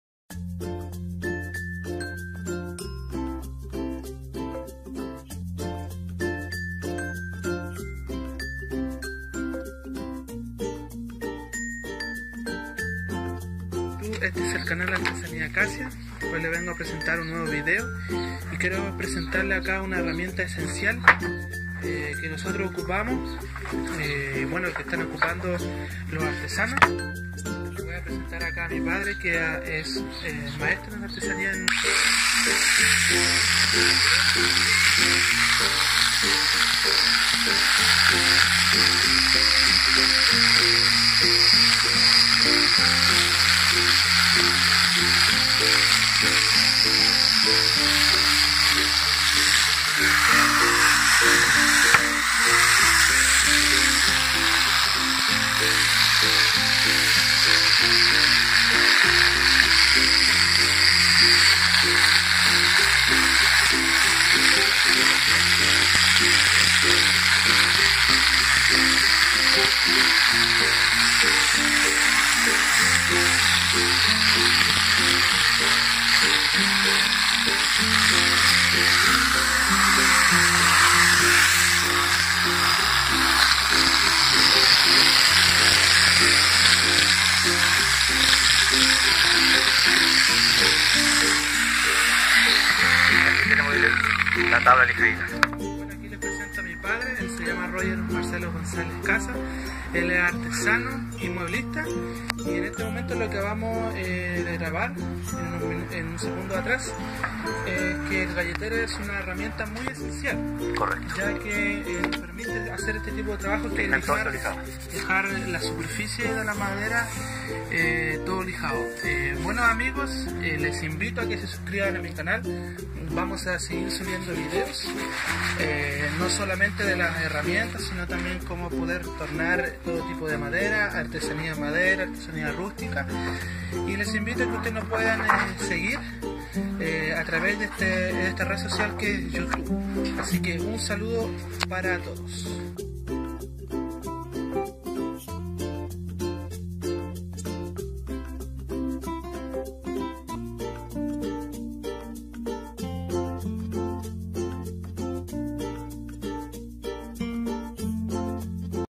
Esmeril mas lija- herramientas esenciales para lijar superficie de maderas
hola amigos aquí les traigo una demostración de un esmeril mas lija con la cual podemos utilizar estas dos herramientas para lijar todo tipo de superficie de madera, en el caso de la artesanía nos ayuda bastante esta herramienta por el simple motivo que nos ayuda en la rapidez de lijar las superficies de la madera y así avanzar mas rápidos en nuestros trabajos